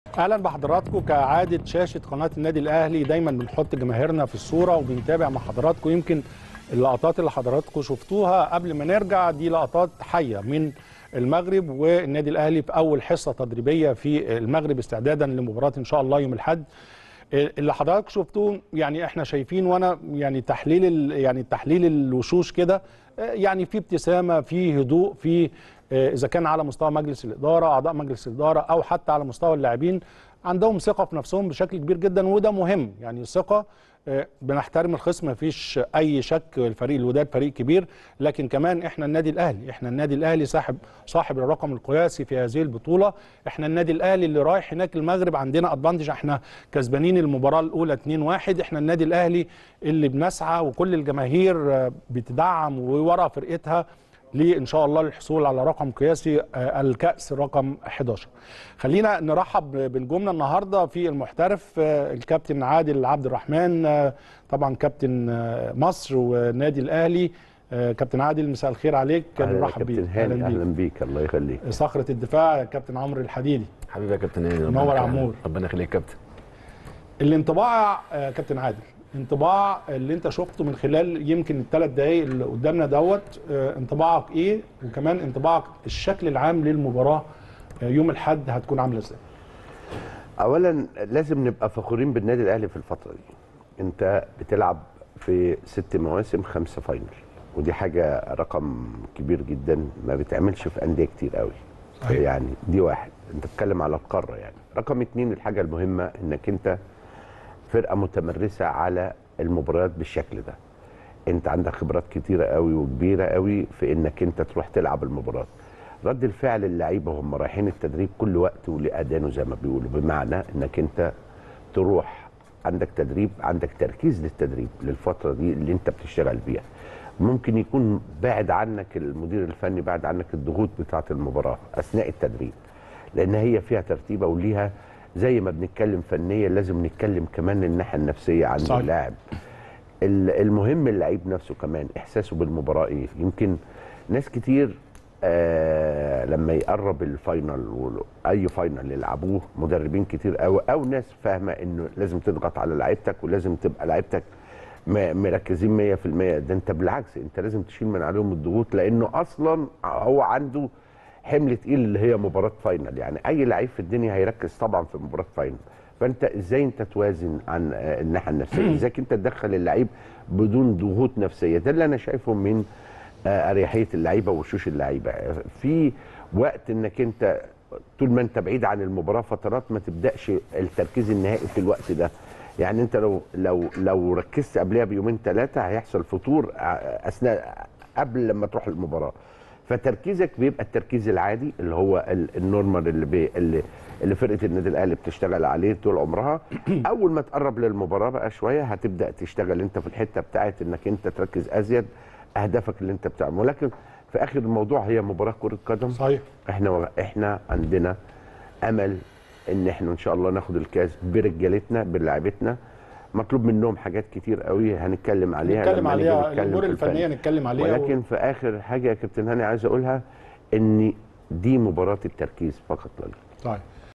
عادل عبد الرحمن: الأهلي متمرس إفريقيا ويمتلك خبرات تؤهله للفوز علي الوداد والتتويج بدوري الابطال
عادل عبد الرحمن: الأهلي متمرس إفريقيا ويمتلك خبرات تؤهله للفوز علي الوداد والتتويج بدوري أبطال افريقيا